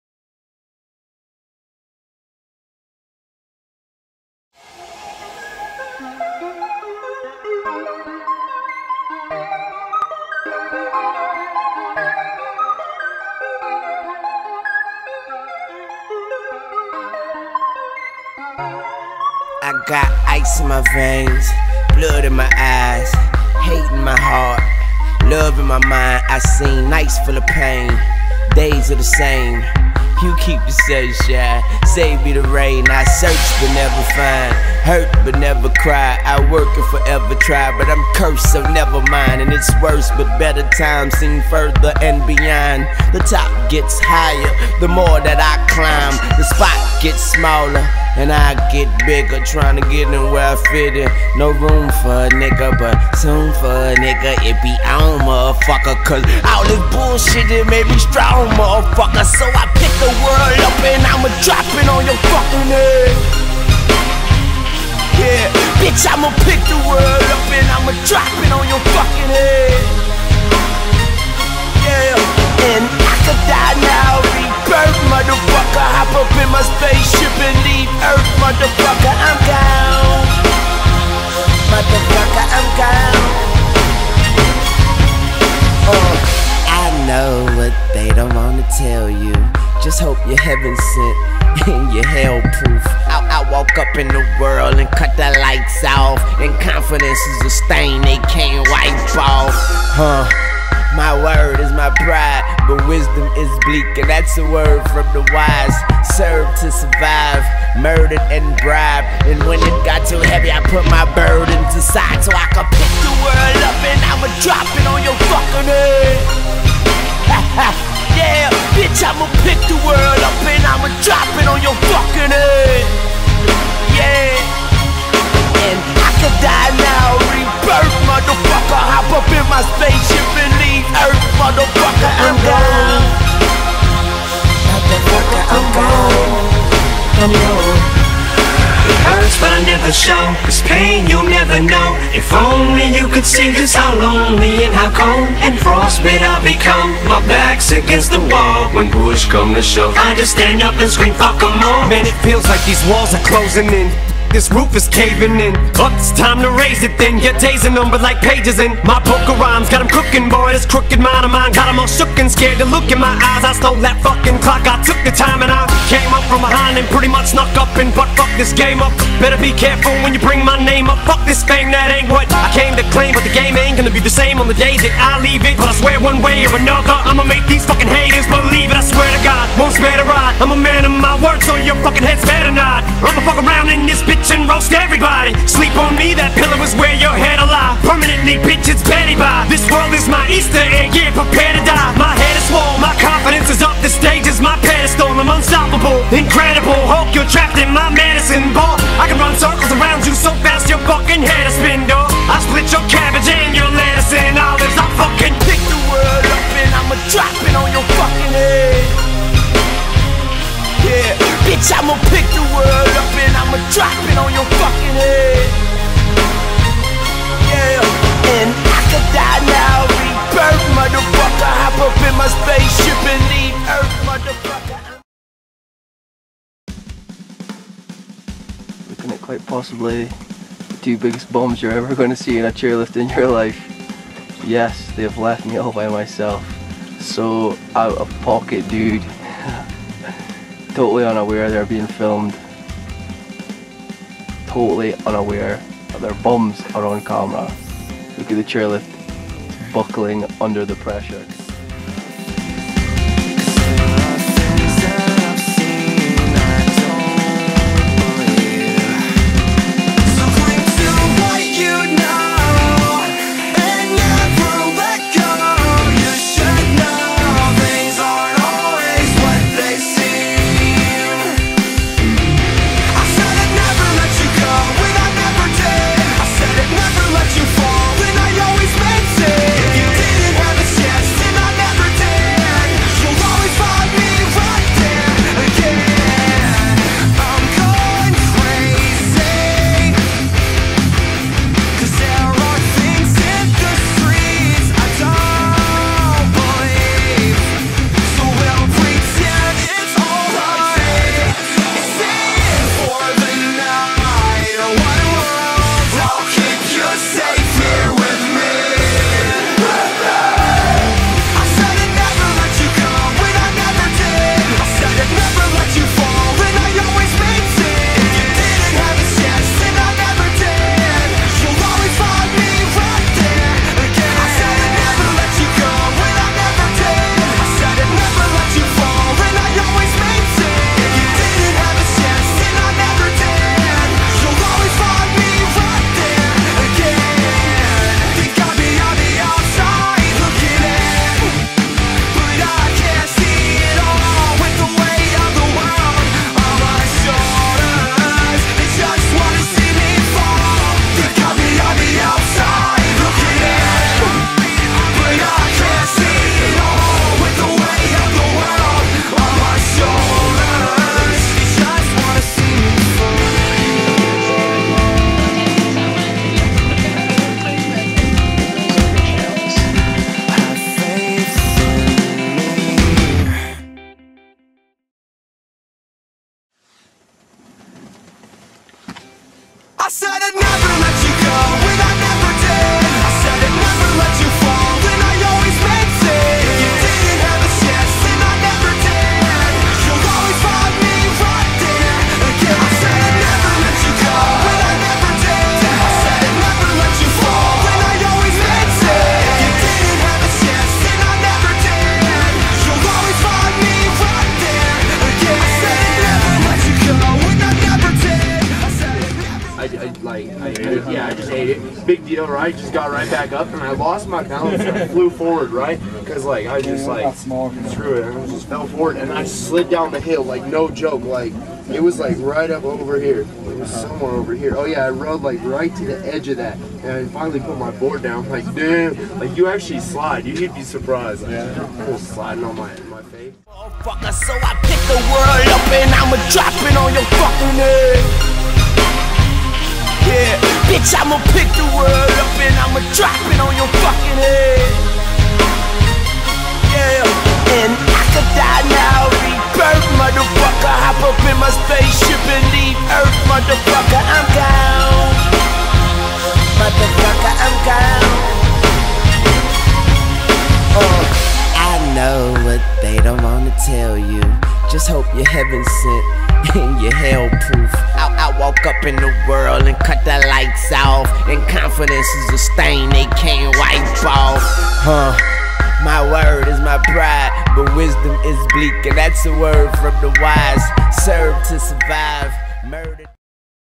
Boarding at Lake Tahoe 2010
Short vid of my holiday to Lake Tahoe, amazing place!